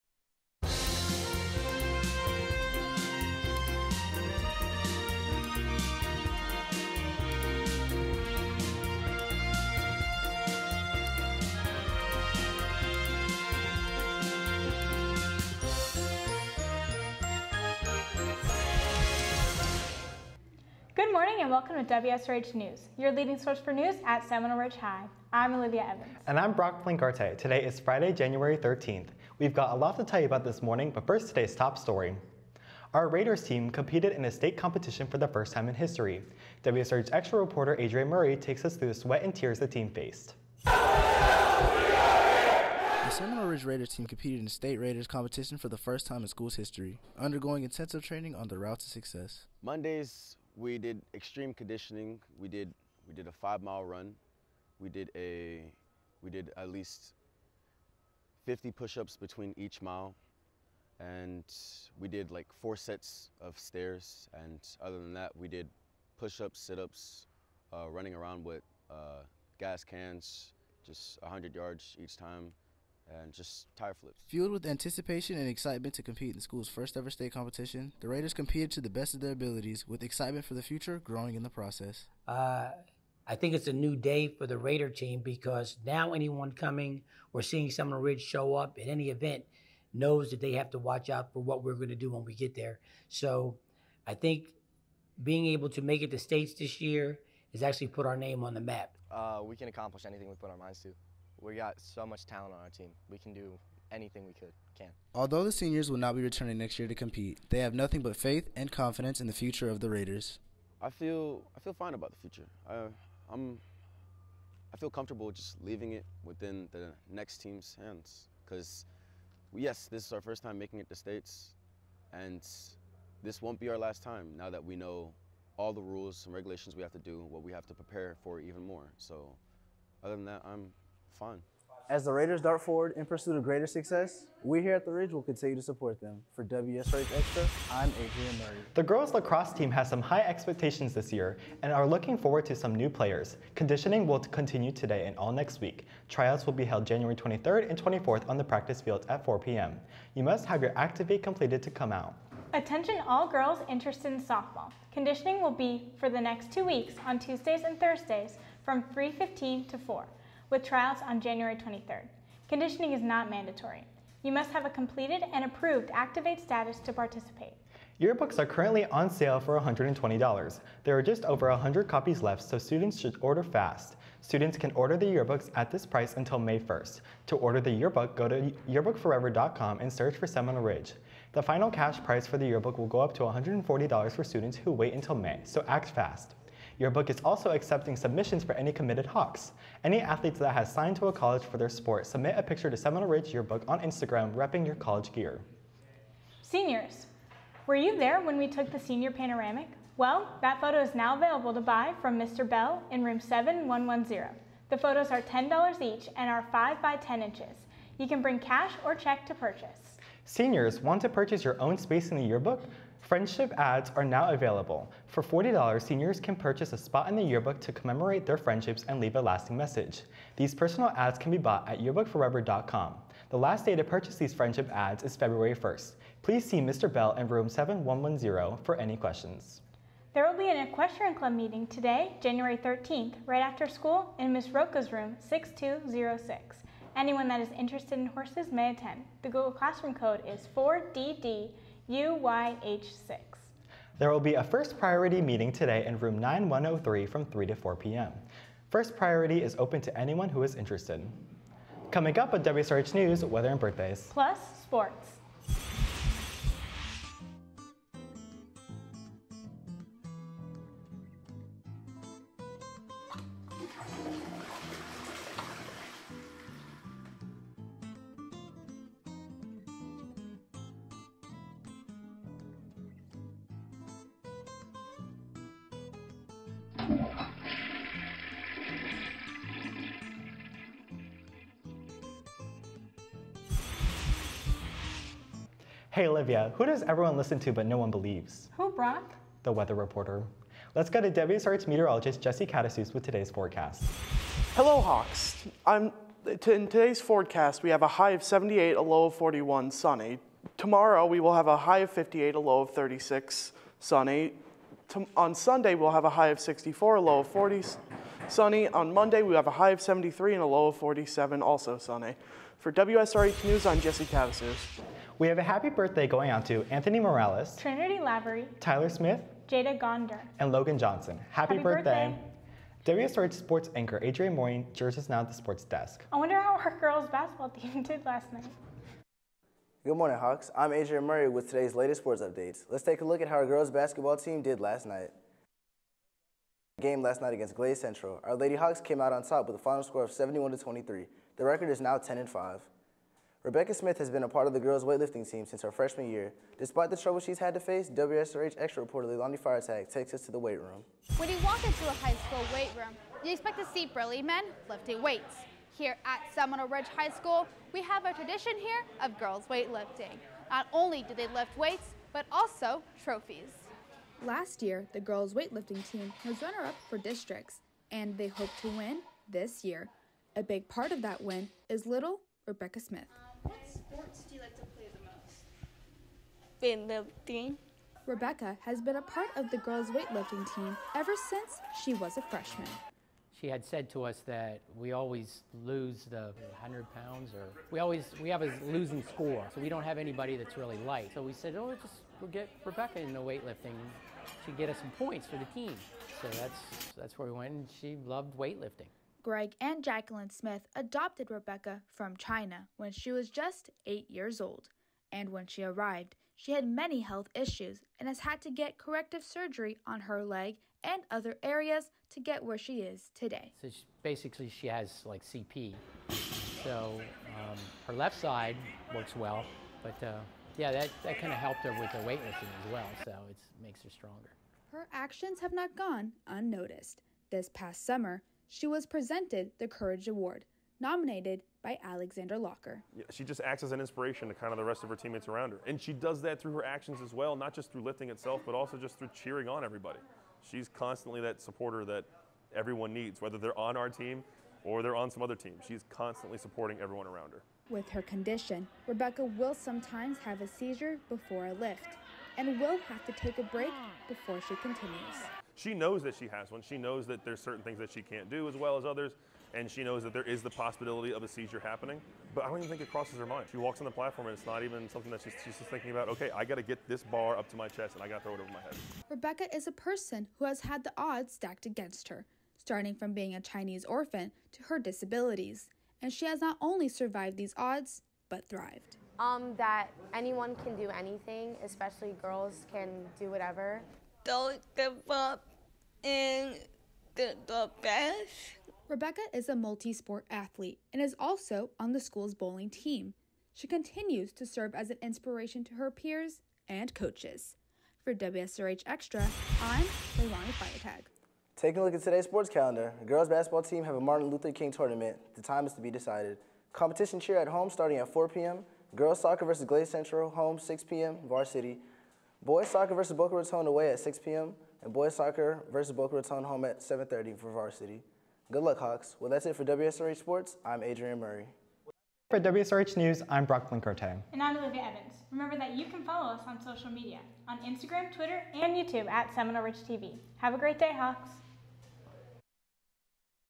WSRH NEWS for 1/13/2023
This is a daily newscast by Seminole Ridge High School Television Production Academy students.